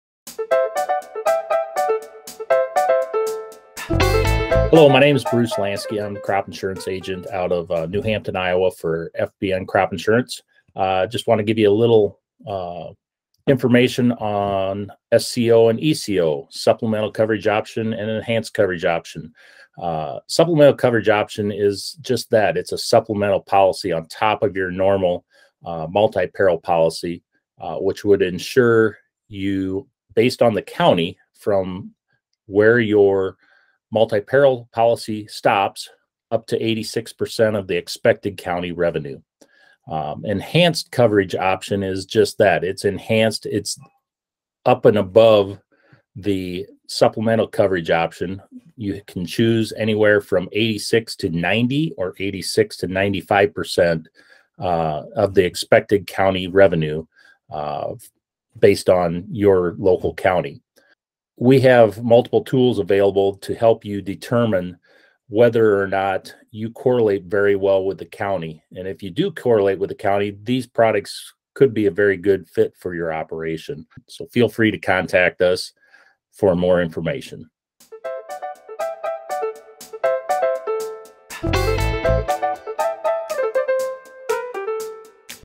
What Are the Differences Between SCO and ECO Supplemental Crop Insurance? | Insurance Intel
Have you ever wondered if supplemental crop insurance is a good fit for you? FBN Senior Crop Insurance Agent Bruce Lantzky walks through the basics of two supplemental crop insurance products: Supplemental Coverage Option (SCO) and Enhanced Coverage Option (ECO). Learn if these may be a good fit for your operation on top of your annual multi-peril policy.
To discuss how this could have an impact on your crop insurance decisions, please